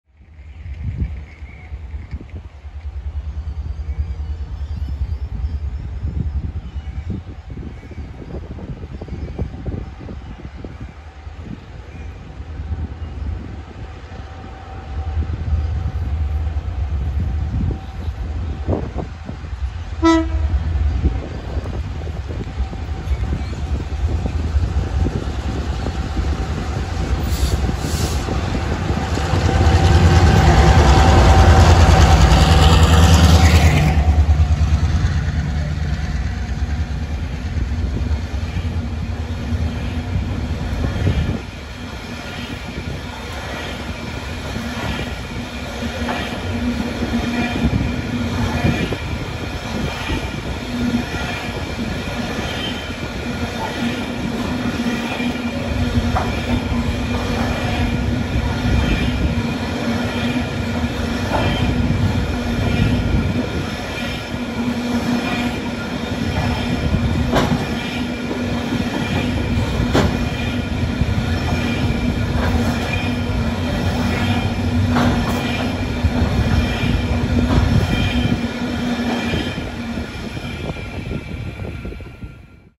Watco Grain through Rosewood
Watco Australia haul grain in WA and Queensland. Here we have WRA001 and WRB2181 with a full load heading towards the port. The WRA class locomotives were purpose-built for the Queensland narrow-gauge network by NRE. The WRB class are ex Queensland Rail 2170 class locos which were on interim service with Transnet in South Africa before being bought by Watco in 2019 and brought back to Australia.